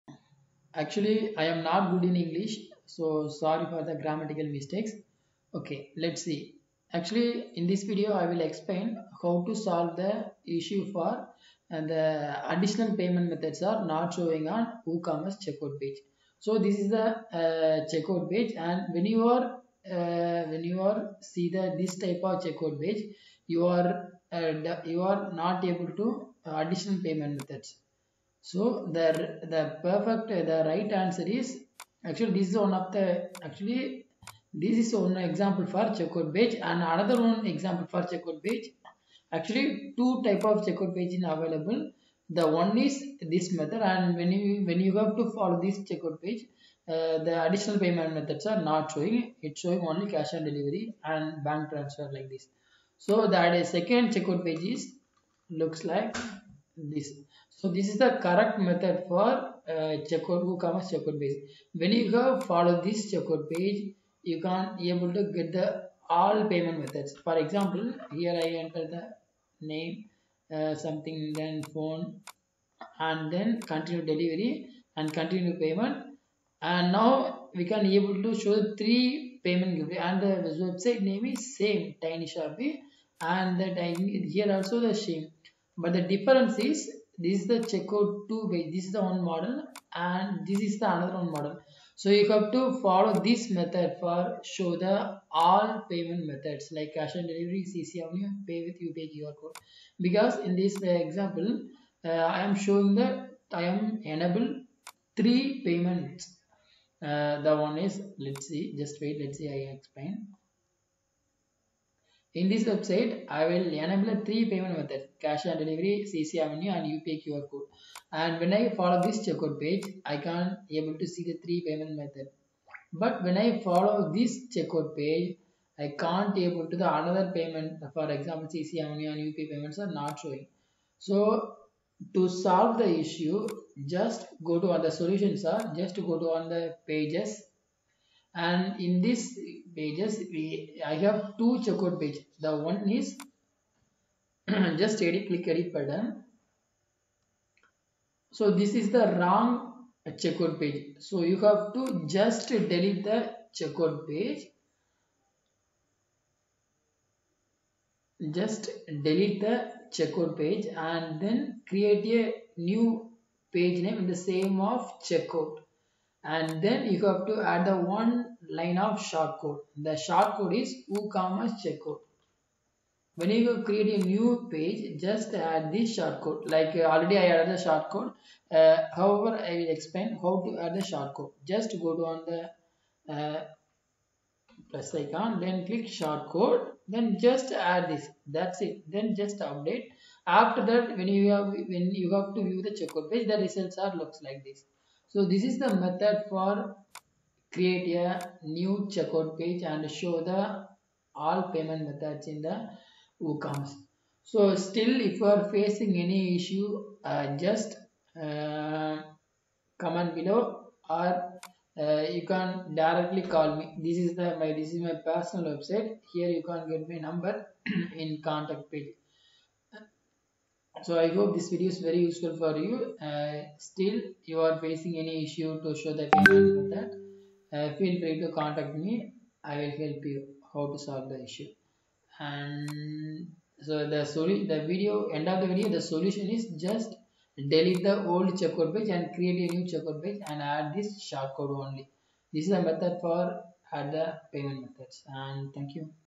Online Payment Methods are Not Showing WooCommerce Checkout Page | WordPress | Problem Solution
In this video I explained how to solve the problem of online payment methods are not showing in WooCommerce ecommerce checkout page in WordPress.